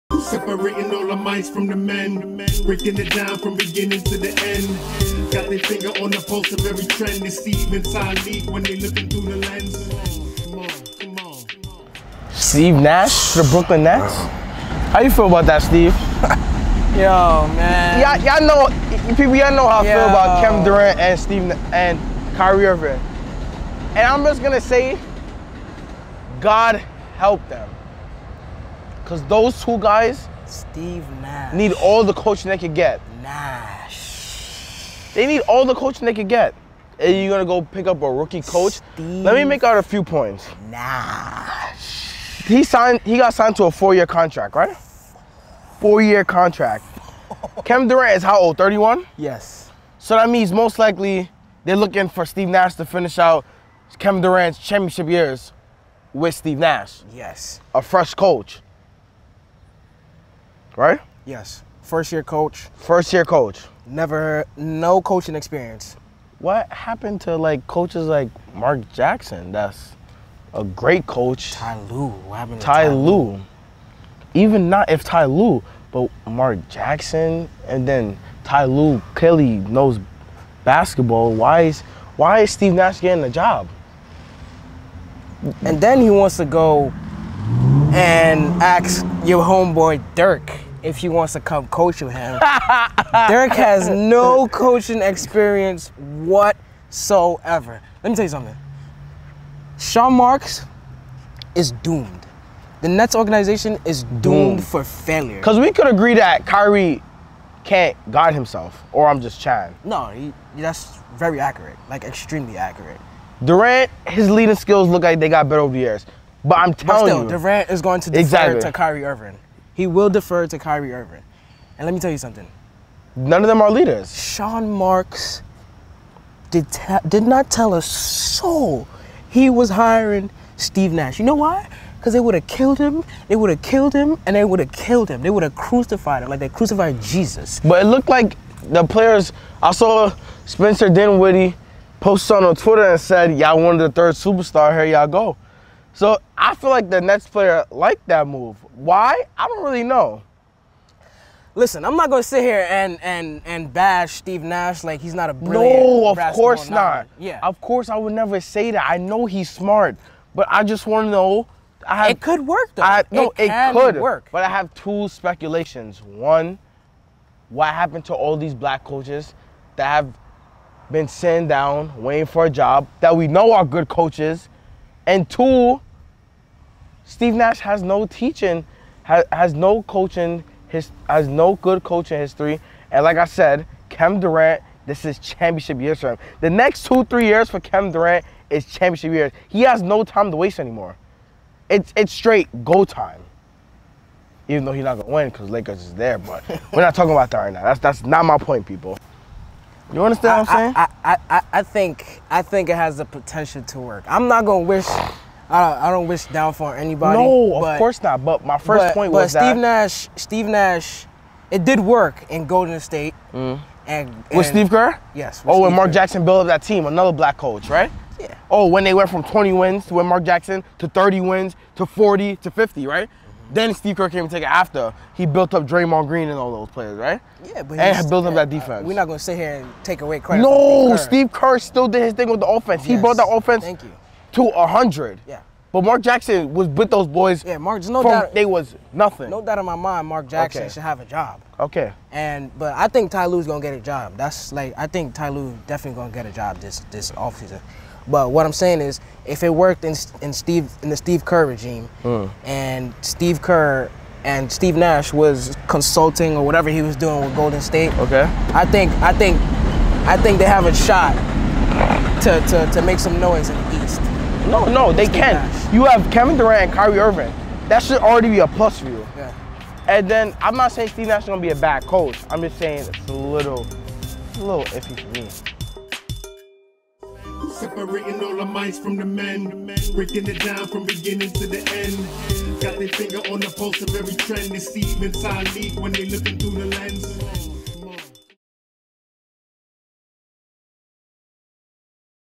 Steve Nash Skips The Line?!??
Thru the Lenz keeps it real about the Brooklyn Nets hire of Steve Nash and the passing on other season coaches. Great points from both Stephen and Talik were made you do not want to miss this one.